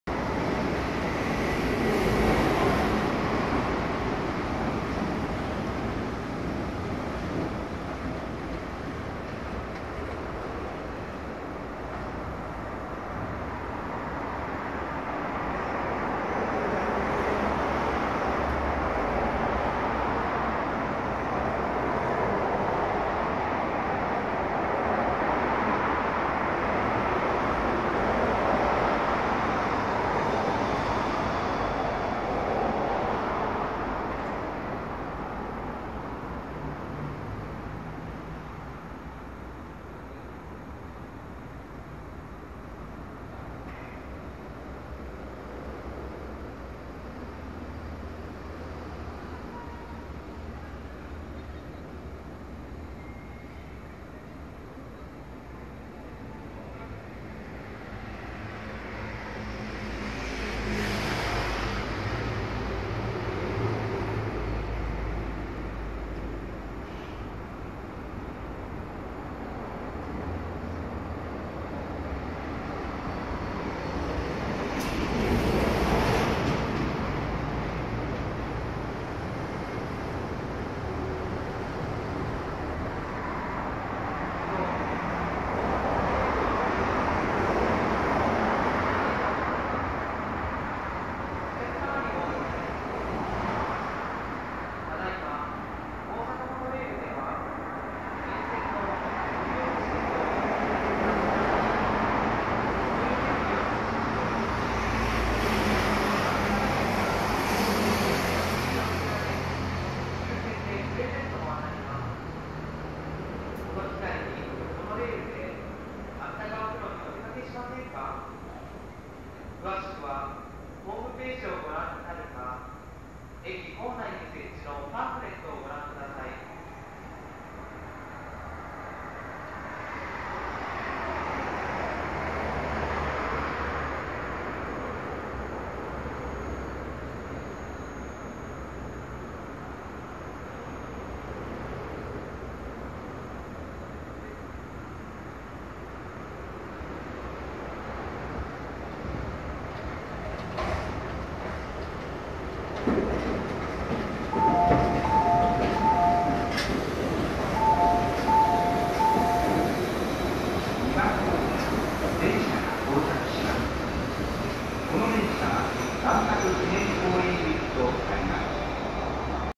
大阪モノレール 彩都西駅 引き上げから進入まで
大阪モノレールの彩都西駅では、乗客を降ろした後、引き上げ線に入り、方向転換して反対ホームに進入する光景を見ることができます。
大阪モノレールでは、門真市駅でも同様の光景を見ることができます。